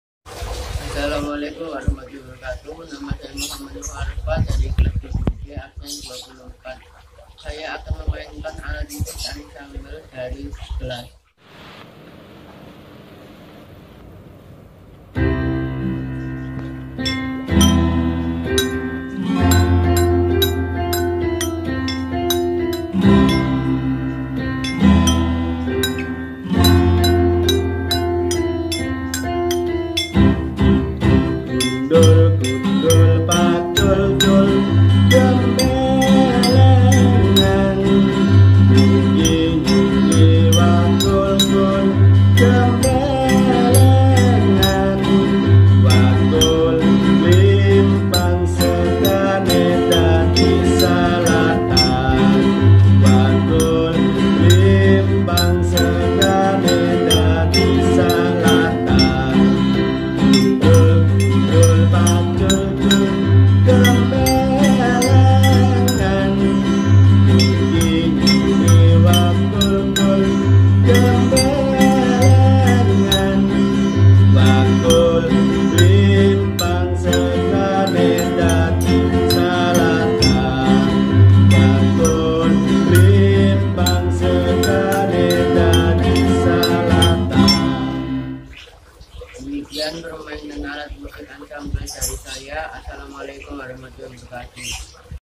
seni budaya kelas 7 - praktik musik ansambel video collage
contoh kreatifitas siswa bermain musik ansambel, membuat video collage dan memainkan alat musik dari gelas dimainkan oleh Muhammad Nucha Arva kelas 7B SMPN 2 Sedati